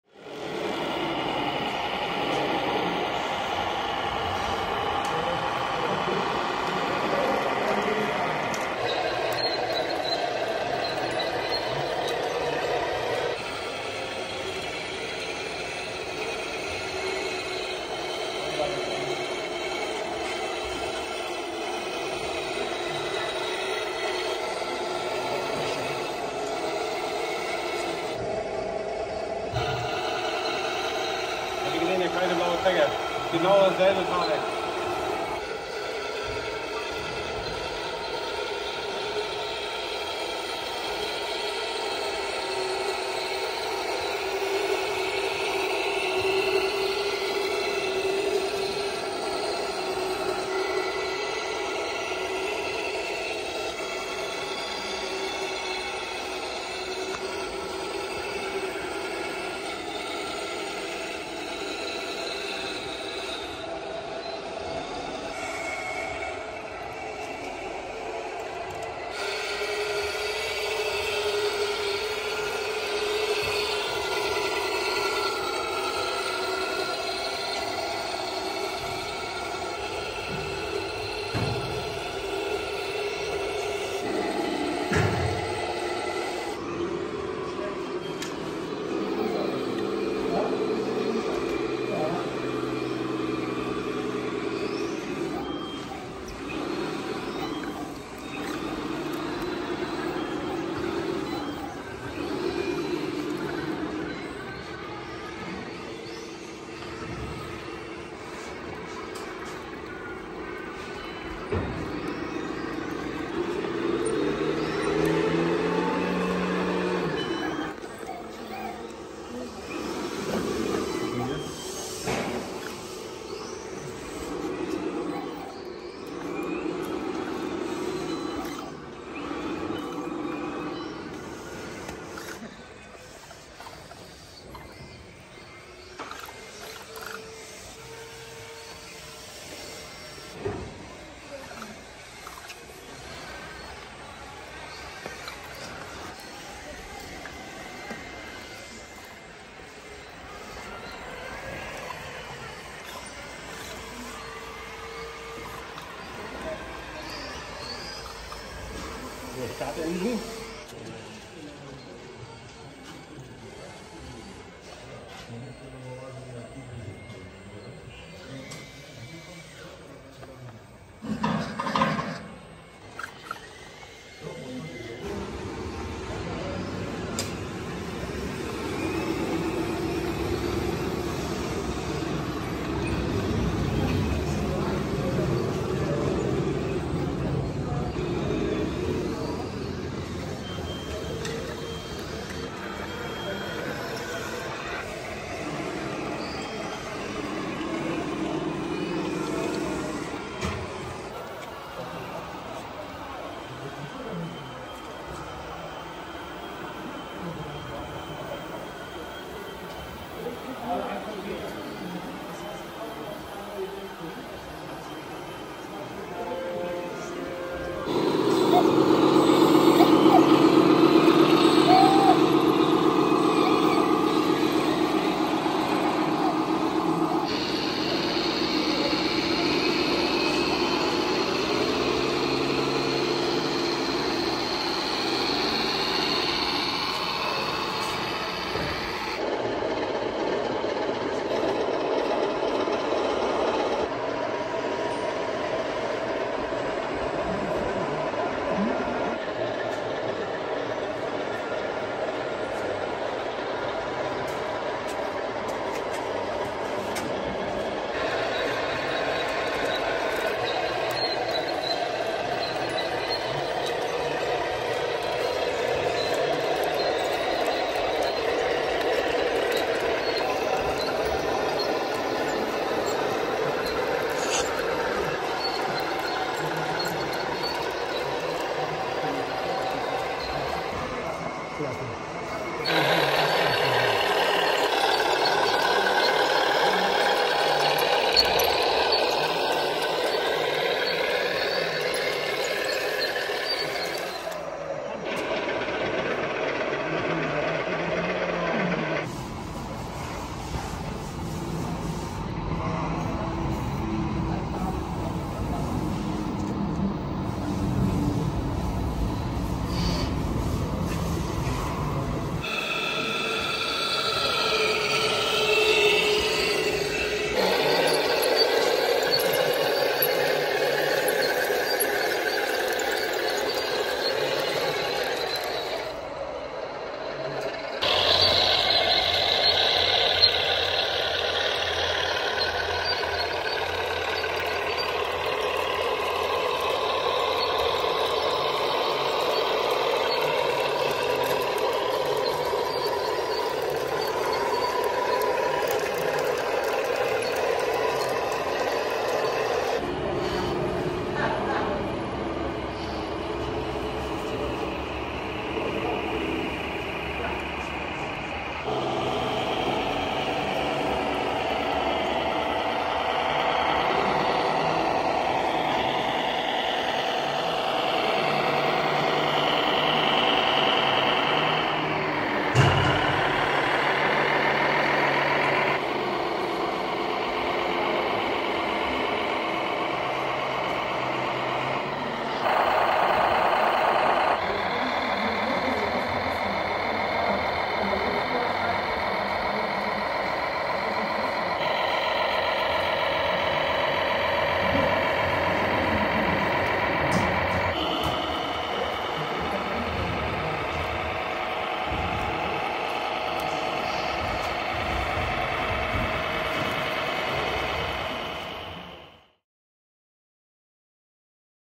MEGA RC Claas Lexion Combine Harvester, RC Tractors, RC Trucks, vintage road repair!
Incredible hand made and modified RC Trucks, Tractors, Claas Lexion Combine Harvester and vintage road construction.
Gefilmt im RC-Glashaus in Quickborn bei Hamburg am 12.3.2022.
Viel Spaß! Enjoy watching!